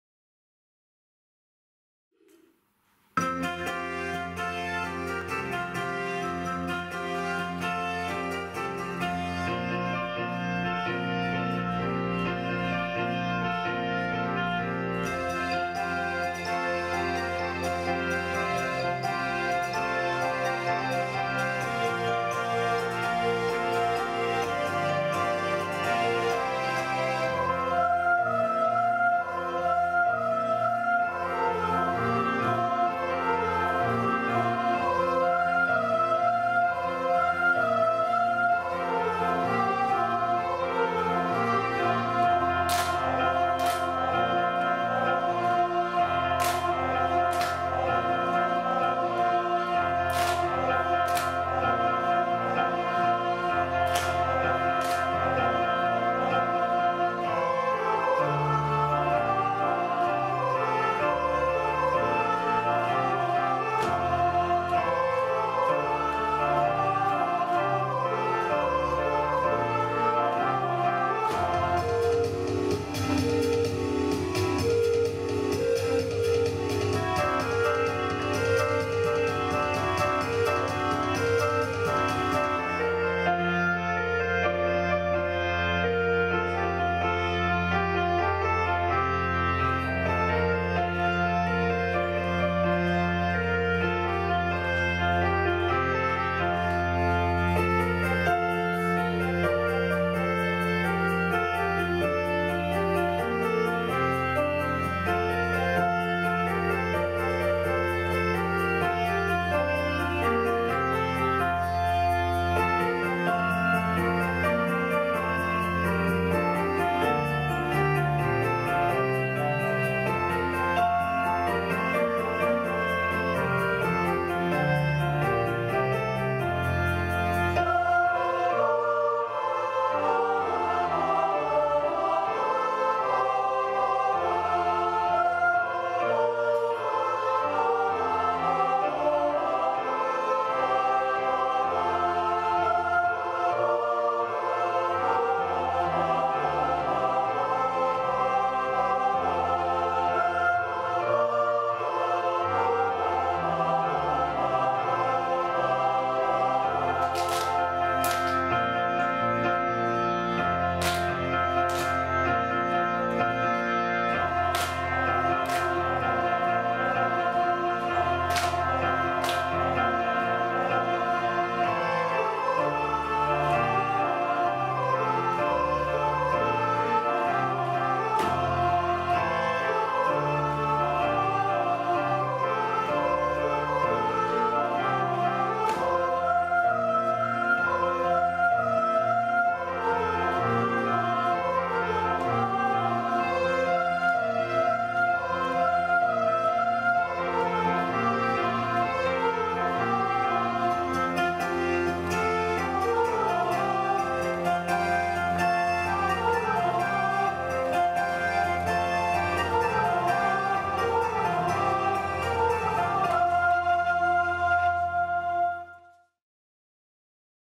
Pirmas šokis
Raseinių Šaltinio progimnazijos jaunių šokių grupė "Versmė". Vadovas - Rimantas Turčinskas.